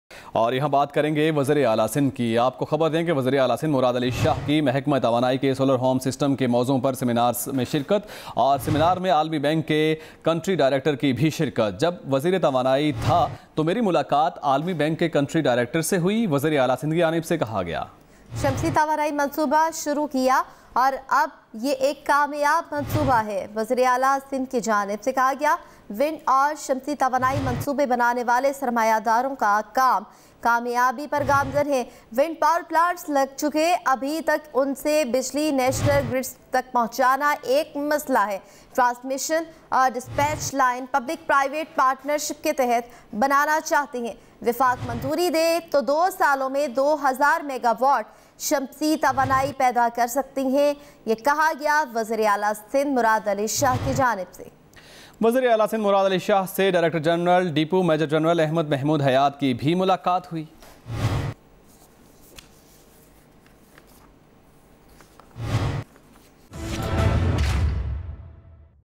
CapitalTV: CM Sindh attends a seminar on Solar Home system in Karachi
Chief Minister Sindh Syed Murad Ali Shah attends a seminar on Solar Home system in Karachi